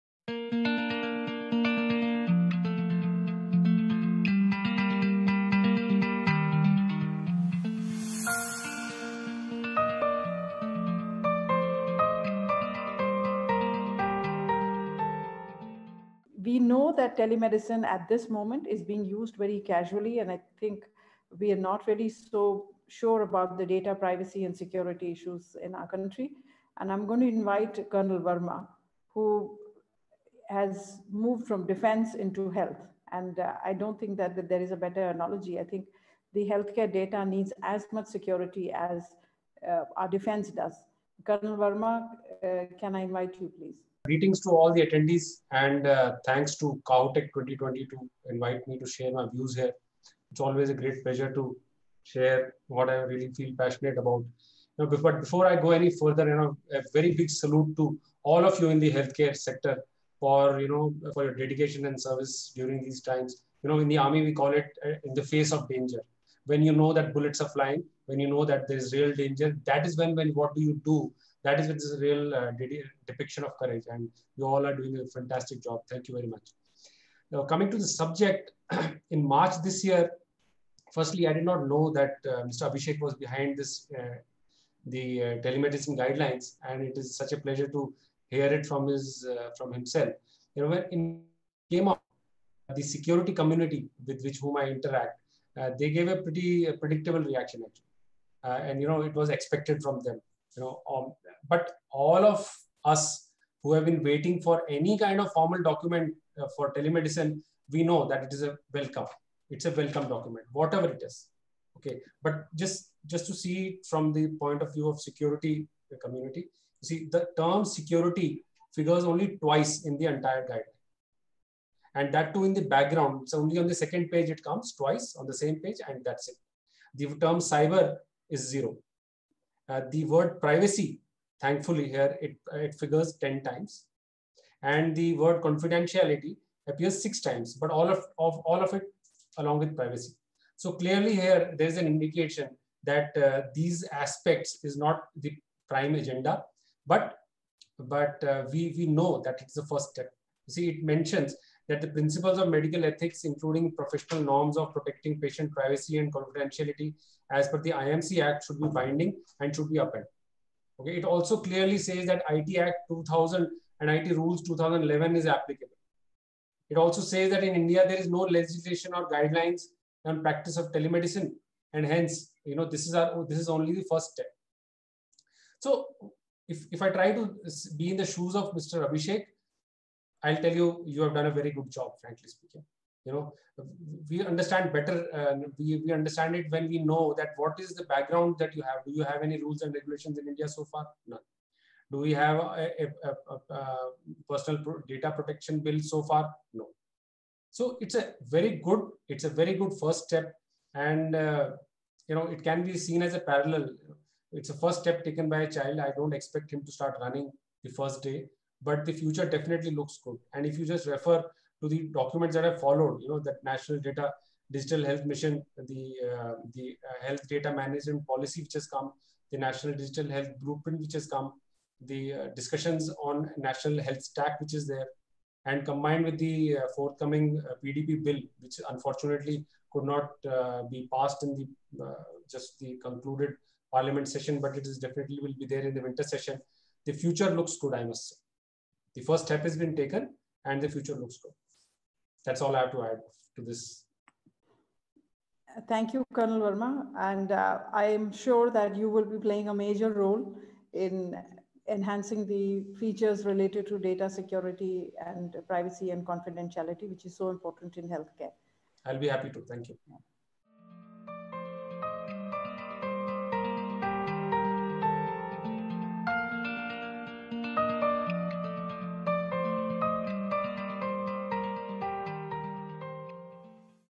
CAHOTECH 2020 (Session 1) : Addressing Challenges in TeleHealth - Cyber Security Perspective
Speaker : Col. Satyendra Verma